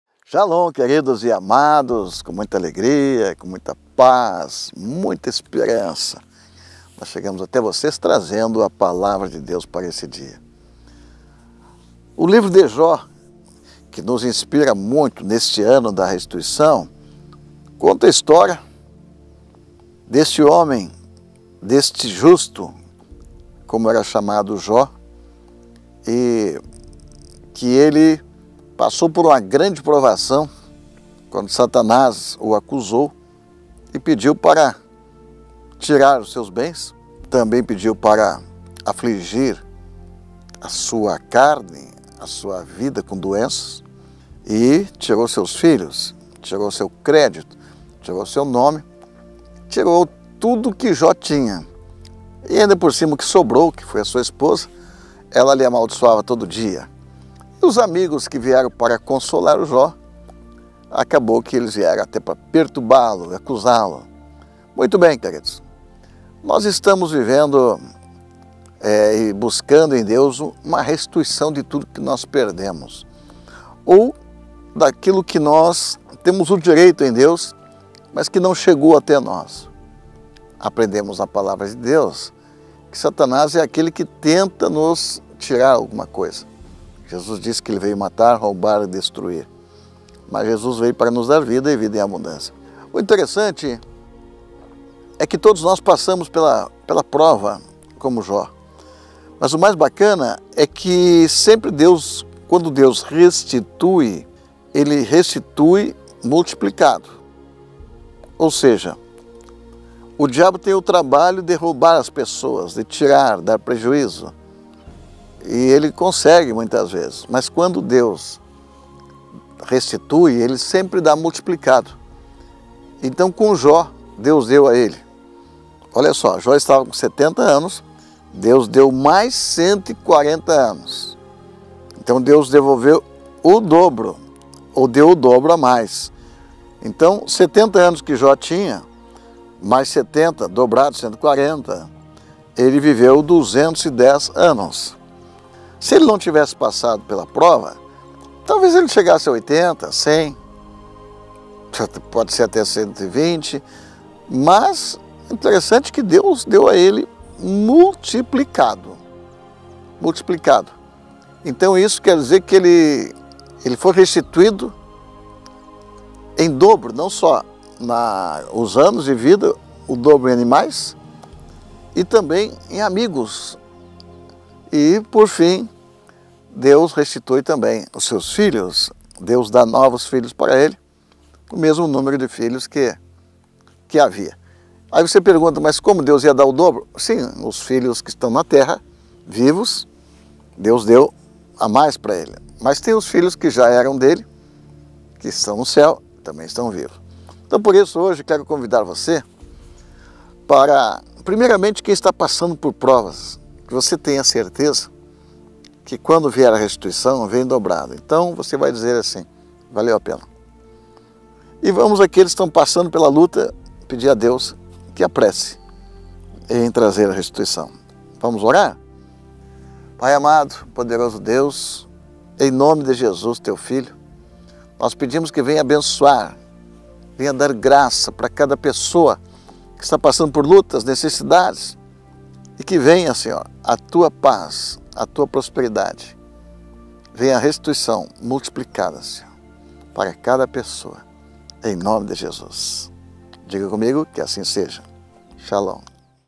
Uma inspiração de fé e restituição
Jó 42

Um homem justo, servo de Deus, que passou por grandes aflições quando foi atacado por satanás, mas que teve um final feliz e próspero. Essa história é a nossa inspiração de fé para 2022. Todos nós passamos por provas, mas temos que entender e crer, que quando Deus nos restitui, Ele o faz de maneira triplicada, abundante, em todas as áreas. Vai valer a pena!